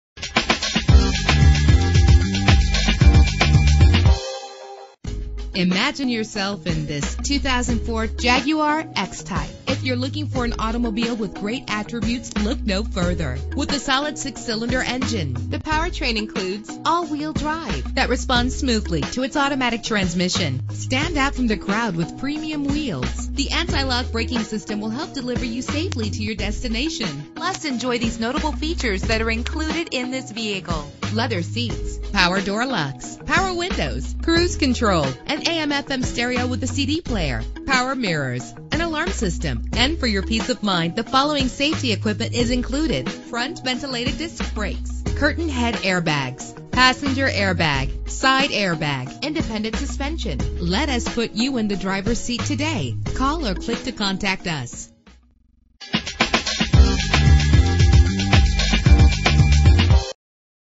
2004 Jaguar X-Type - Winter Haven FL
Year: 2004
Make: Jaguar
Model: X-Type
Trim: 2.5
Engine: 2.5 liter 6 cylinder 24 valve
Transmission: Automatic
Color: Black
Mileage: 70249
Address:
6395 Cypress Gardens Blvd